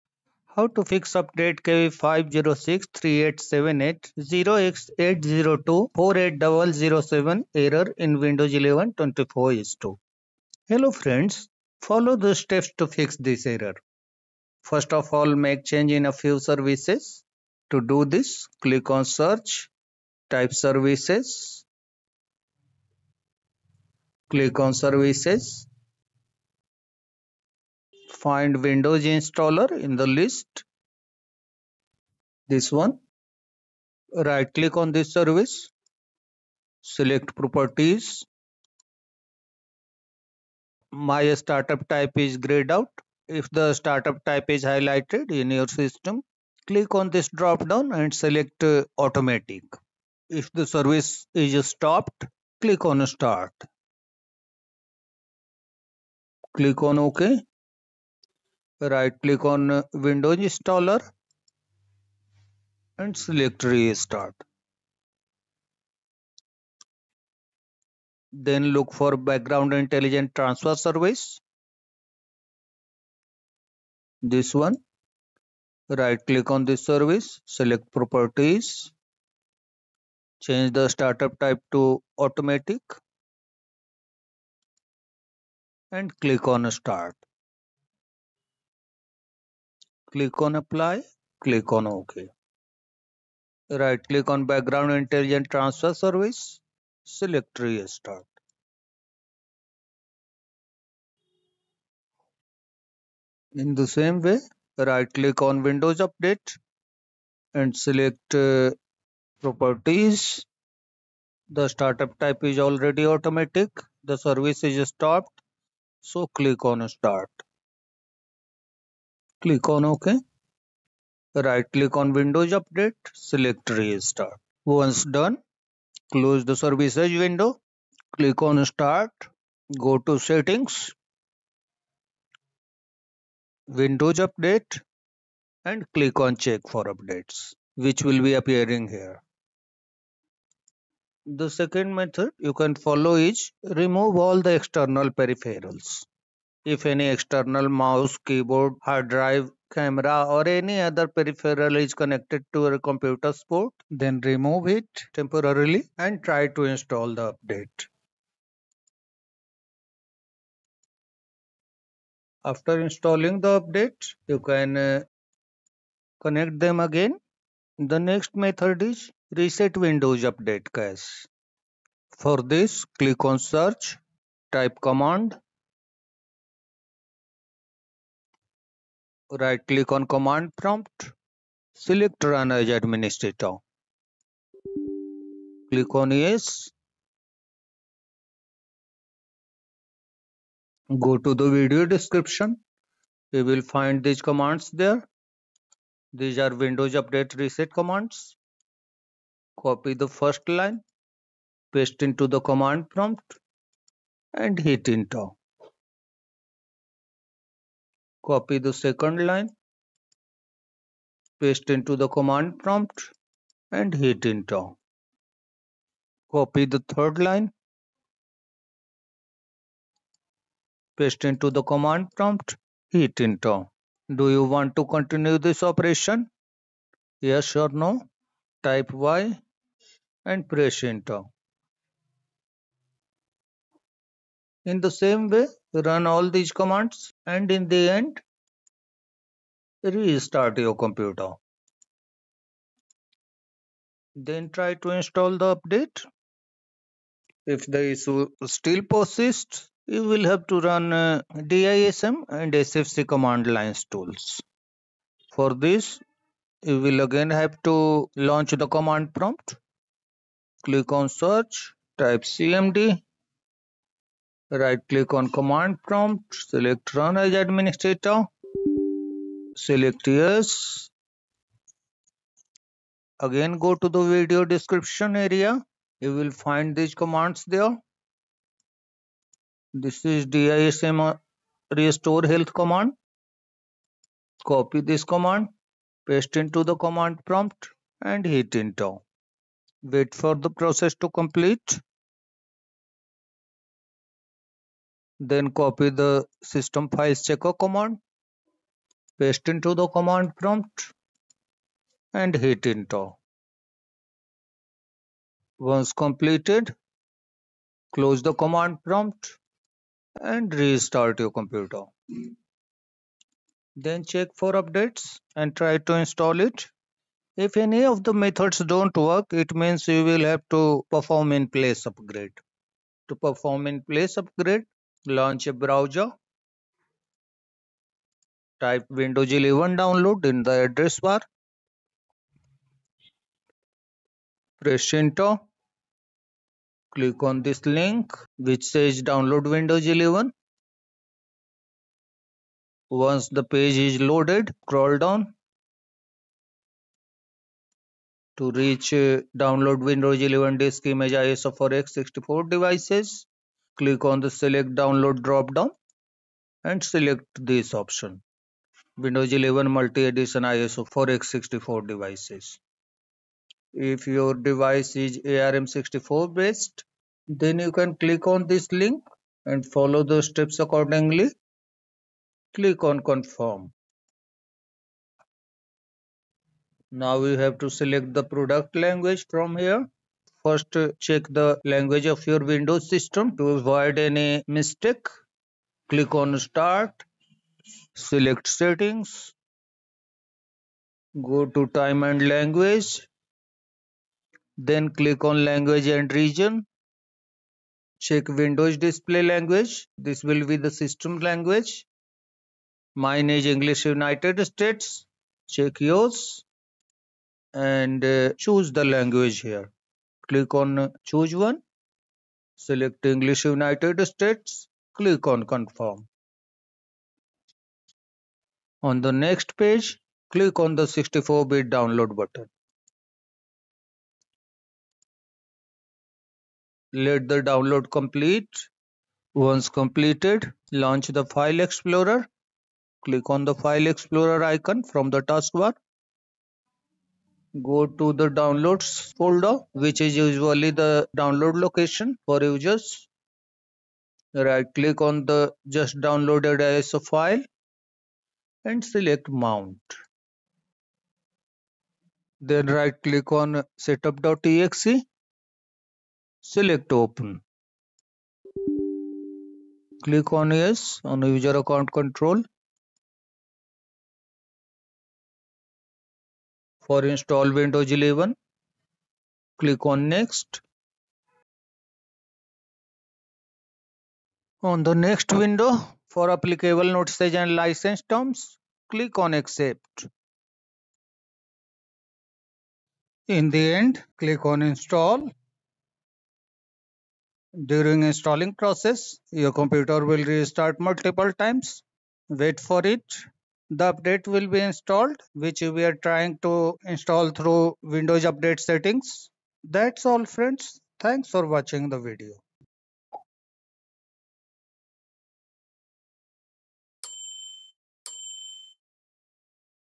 How to fix update KB5063878 0x80248007 error in Windows 11 24H2
ren %systemroot%\softwaredistribution softwaredistribution.bak
ren %systemroot%\system32\catroot2 catroot2.bak

System files checker command:

SFC /SCANNOW